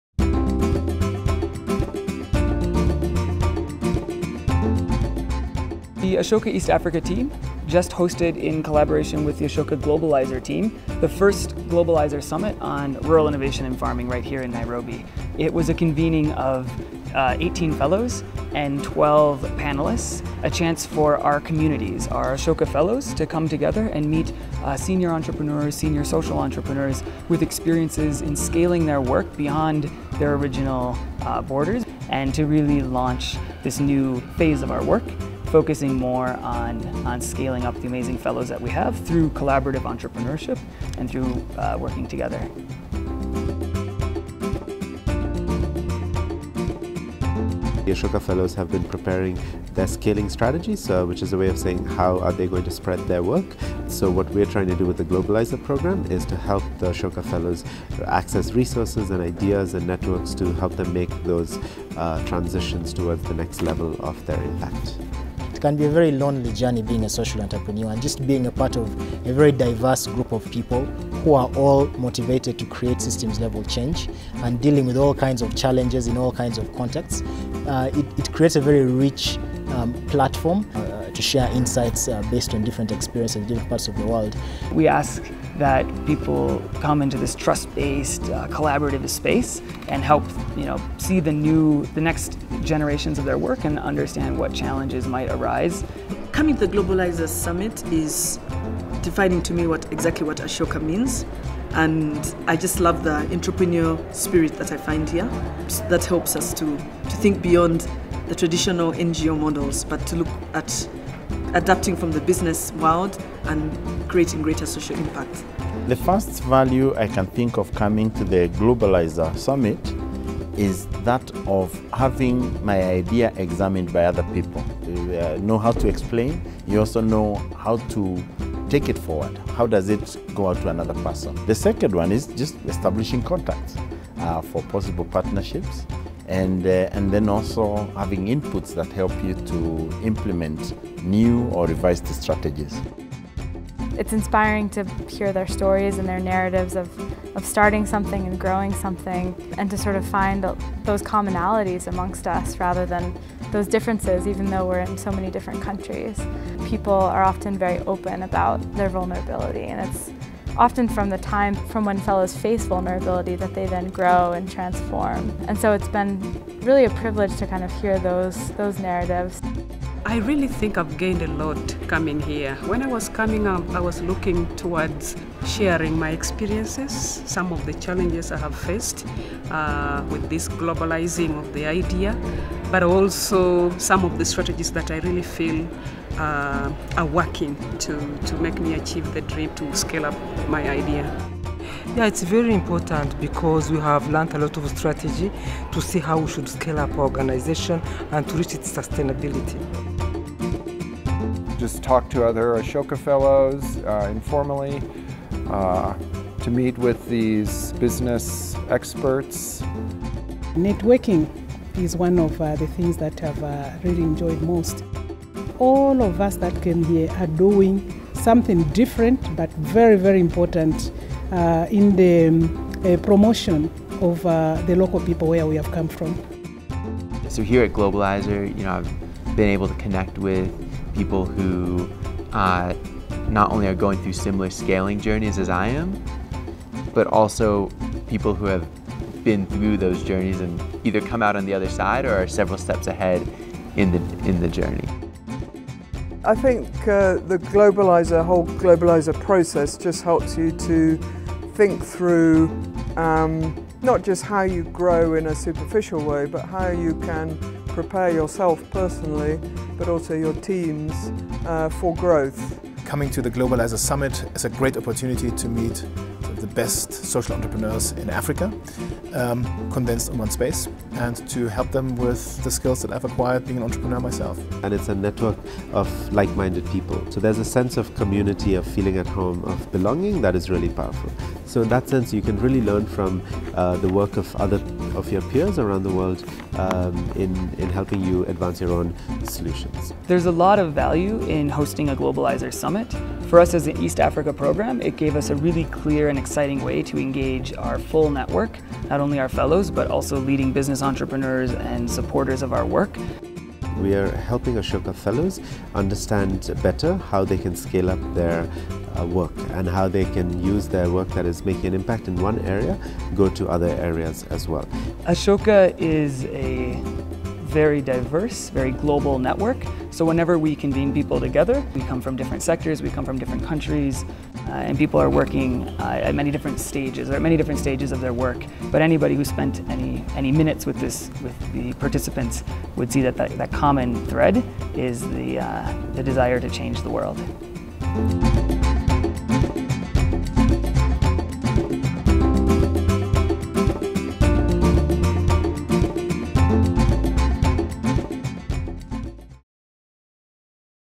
ASHOKA EAST AFRICA - NAIROBI GLOBALIZER SUMMIT
Ashoka East Africa held the Nairobi Globalizer Summit on Rural Innovation on